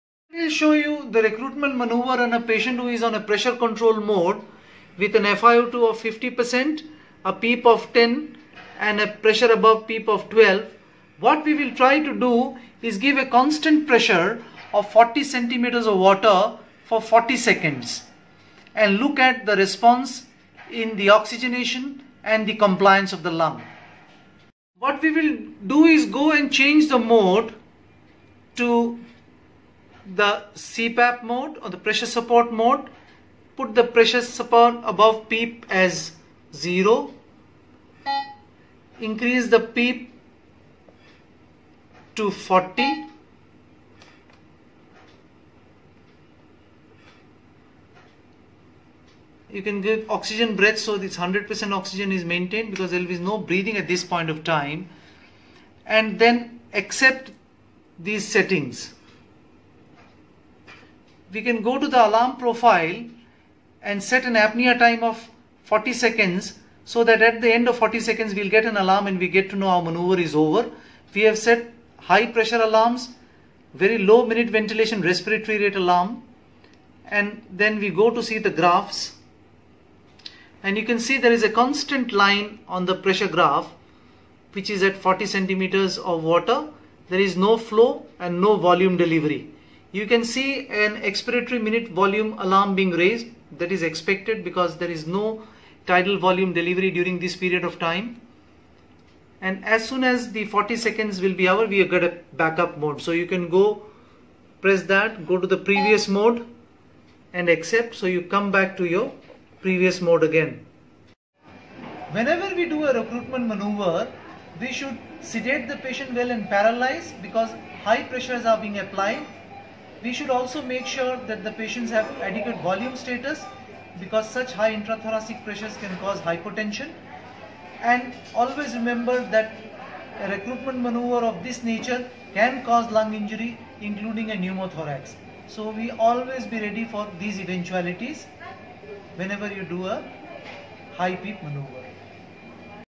RECRUITMENT MANEUVER: Real Time
Giving a CPAP of 40cm H2O for 40sec